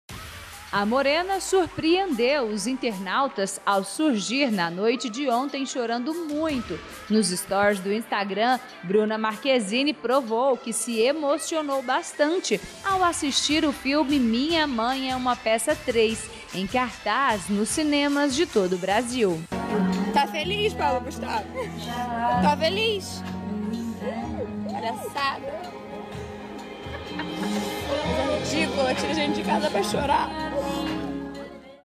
Bruna Marquezine surge aos prantos no cinema "Tá feliz agora ? "
Bruna Marquezine não segurou a emoção e foi às lágrimas ao assistir ao filme Minha Mãe É Uma Peça 3 na noite desta quarta-feira, 8, no Rio de Janeiro.

Na sala de cinema, ela gravou um vídeo para Paulo Gustavo, idealizador e protagonista do filme. "Está feliz, Paulo Gustavo? Palhaçada! Coisa ridpicula, tira a gente de casa pra chorar", brincou ela enquanto secava as lágrimas.

"Parabéns, parabéns, parabéns. Está divertidíssimo e lindo! Me emocionei várias vezes. Você é genial e inspirador", derreteu-se ela. "Eu sei que você já desistiu de mim porque eu nunca vou na sua casa quando você chama, mas eu te adoro e sou sua fã! Me dá mais uma chance", brincou ela.